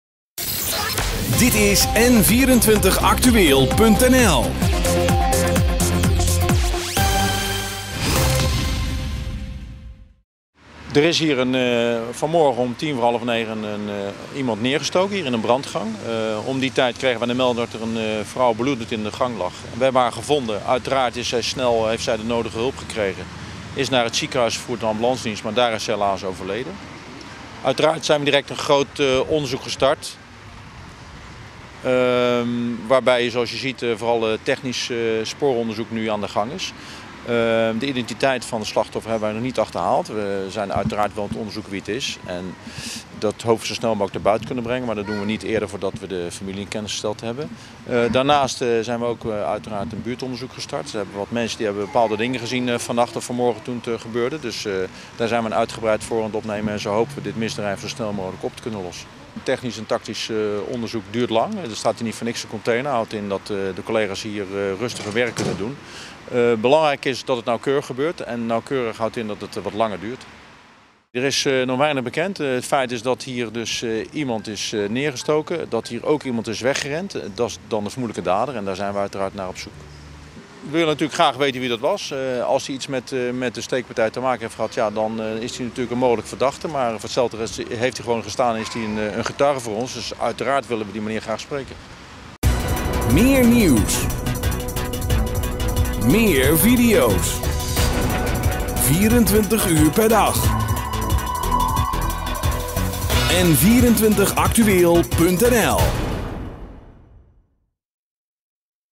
N24actueel - Politie Den Bosch druk bezig met onderzoek naar dodelijke steekpartij
De politie is in de omgeving van de Leonardus van Veghelstraat in Den Bosch al de hele ochtend bezig met een uitgebreid sporenonderzoek. Rond half negen werd in een brandgang een 19-jarige vrouw uit Zweden door een onbekende man neergestoken.

De vrouw raakte bij de steekpartij zwaargewond. Het slachtoffer is nog naar het ziekenhuis gebracht, maar daar is ze overleden aan het verwondingen.

Omwonenden zeggen dat er zich tussen 7.00 uur en 8.30 uur een verdachte man heeft opgehouden in de buurt van de Leonardus van Veghelstraat. De man droeg een capuchon en was nogal zenuwachtig.

Volgens politiewoordvoerder Eric Passchier van de politie in Den Bosch kan de zenuwachtige man de verdachte van de steekpartij, maar ook een getuige zijn. Volgens Passchier doet de technische recherche nauwkeurig onderzoek naar de sporen in de buurt van de plek waar de Zweedse vrouw dodelijk is verwond. "We hebben niet voor niets vanmorgen hier een politiecontainer geplaatst", aldus Passchier tegen N24actueel. Politiehonden speuren de omgeving van de 'plaats delict' af.

De vrouw uit Zweden was volgens getuigen op bezoek bij bekenden die in Den Bosch wonen.

--- Nieuws uit de regio Den Bosch ***